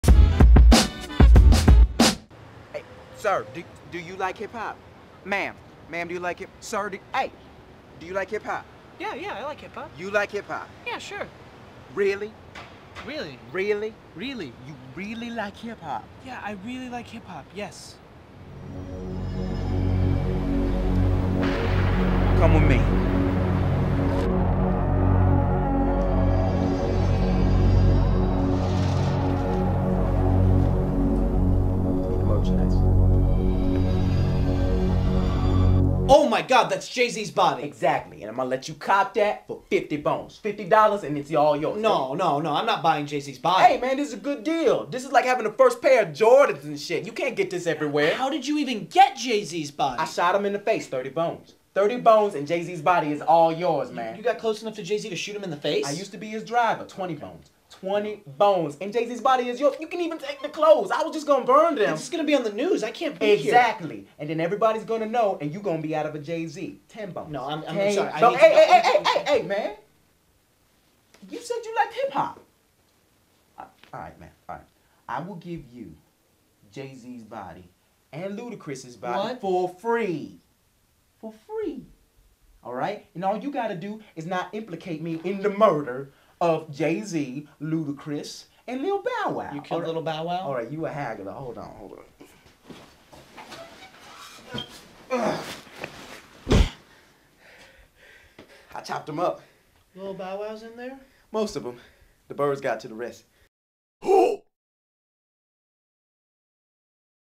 Derrick's "Do You Like Hip Hop"
CollegeHumor presents the first of its original videos by Derrick Comedy.